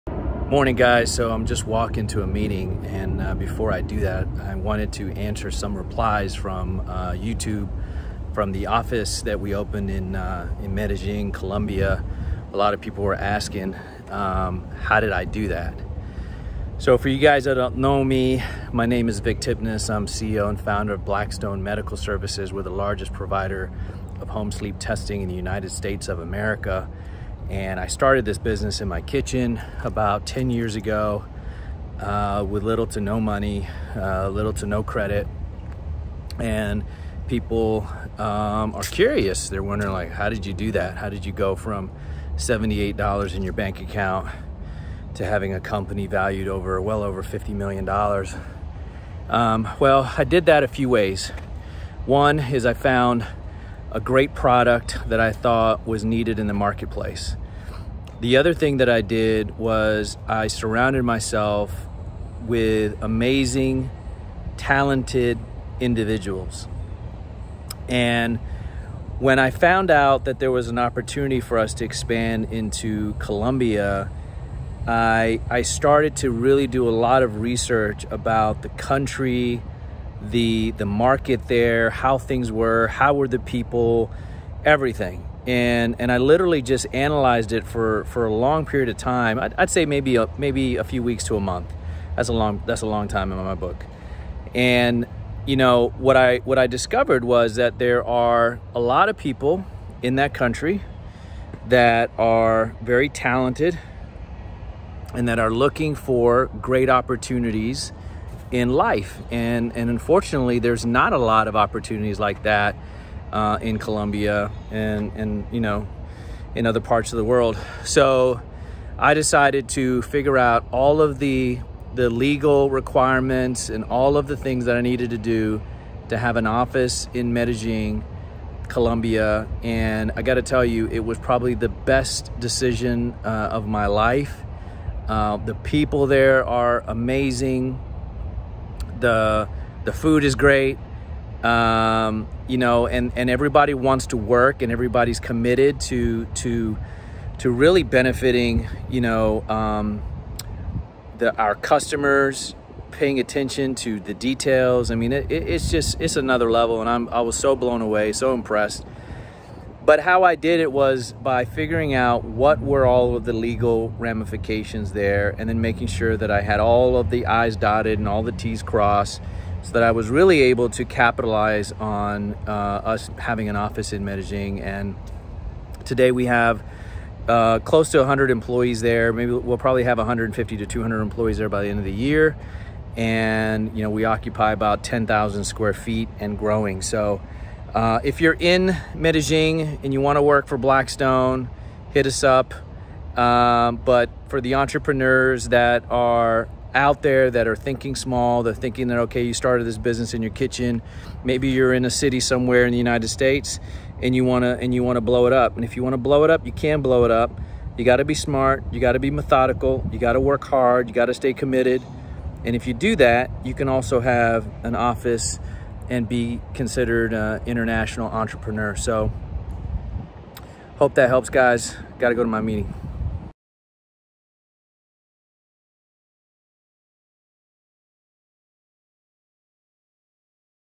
How to invest in another country?
Does your company have the potential to become international? In this video I will give you tips that I used, to expand your company in a country like Colombia.